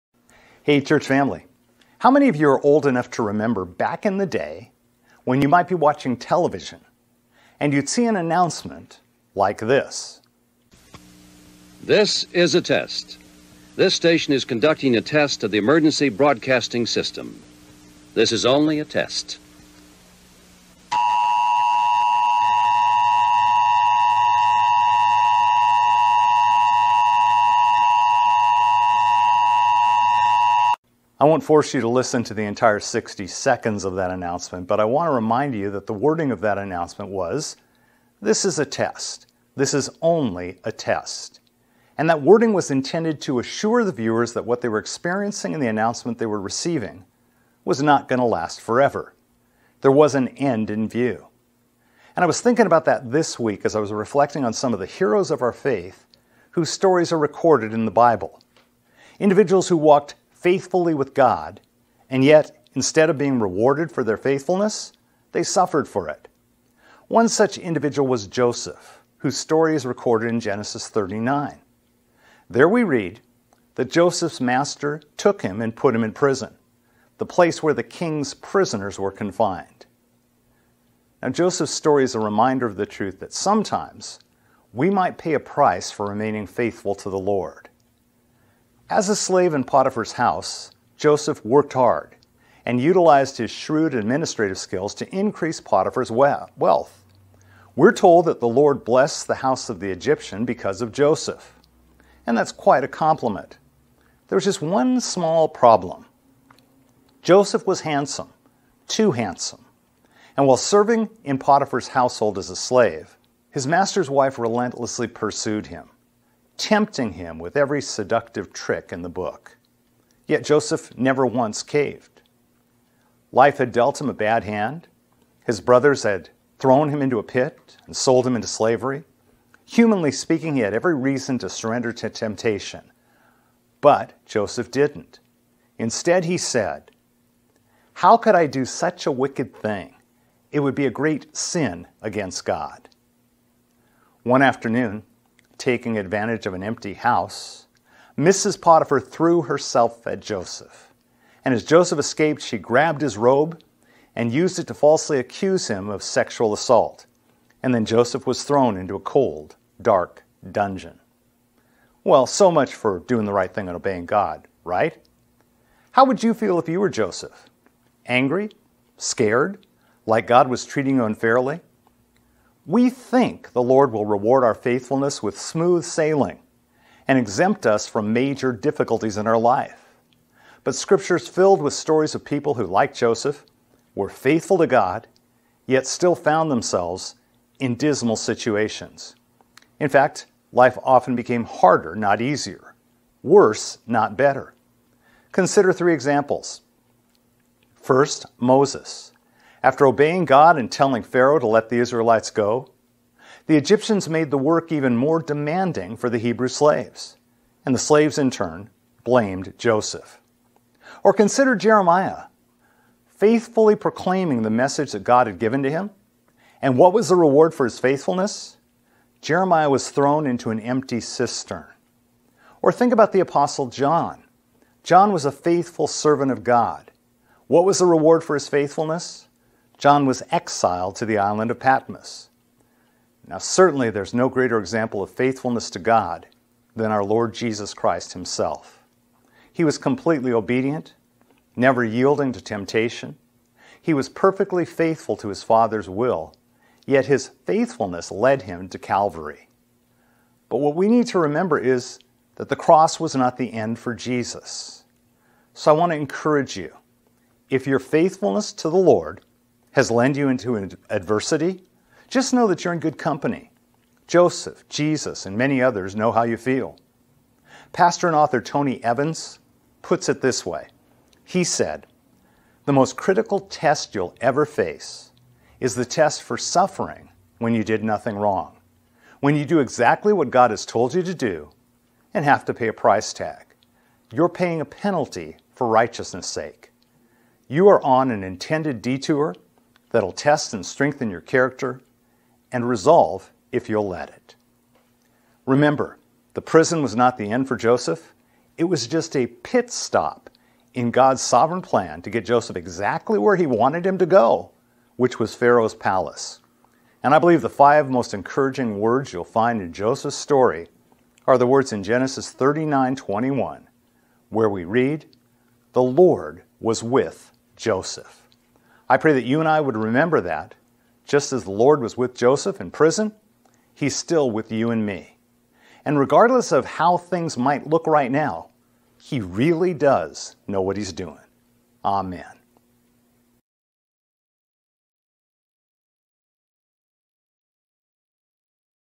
This Is A Test, This Is Only A Test: Video Devotional By Dr. Rick Mandl
Pastor Rick looks at Joseph's story in Genesis 39 and talks about what happens when our faithfulness to God results in suffering for us.